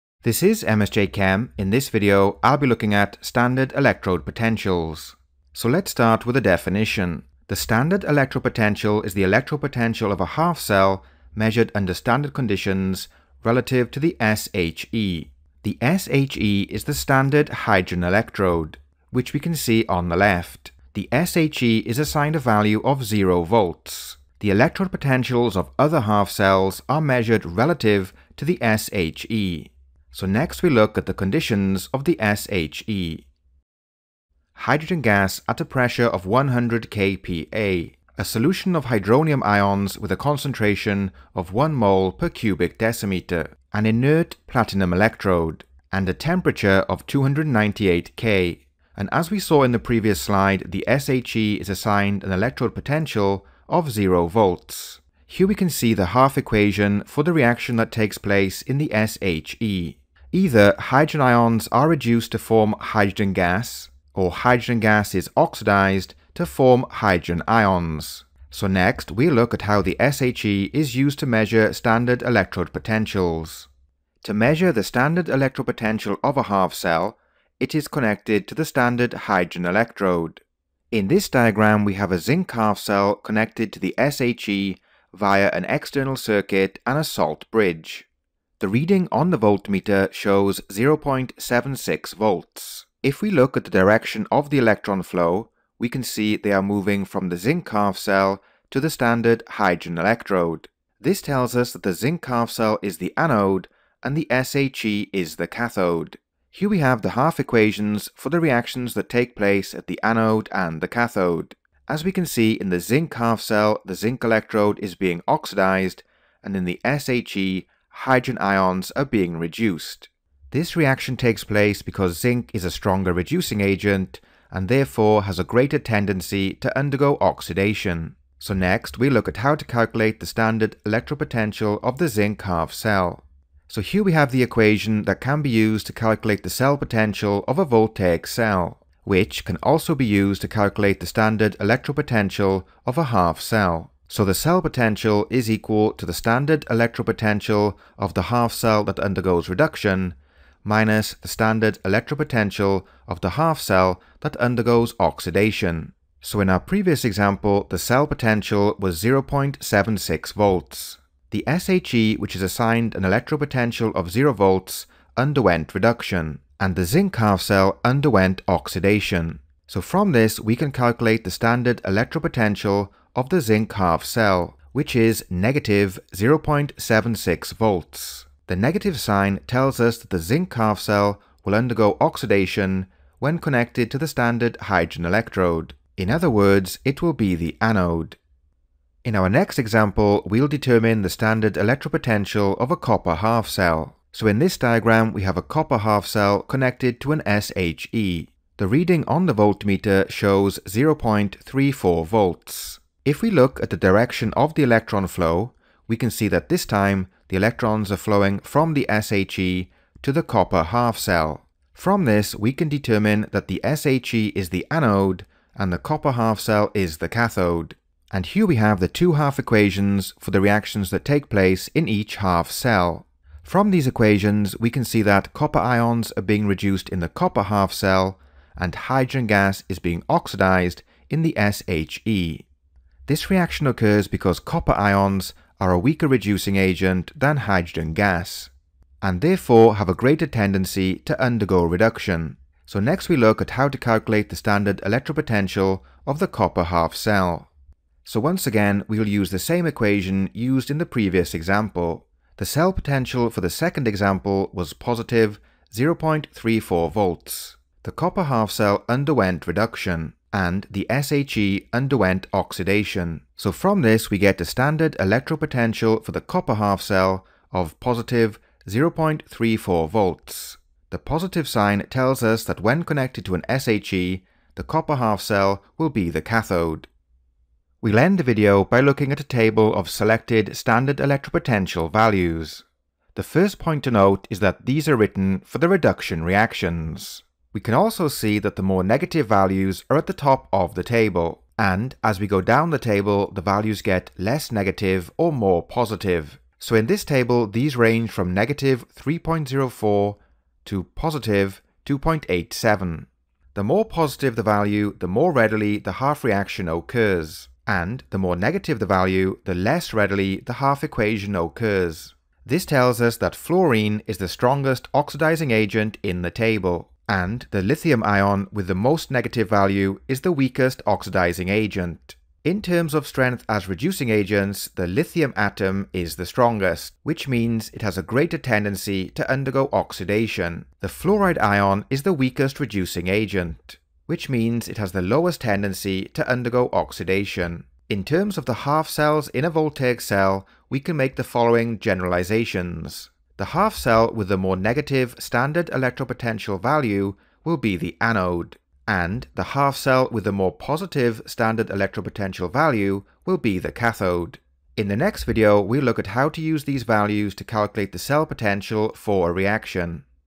R3.2.12 Standard electrode potentials (HL)
This video covers the hydrogen half-cell (SHE).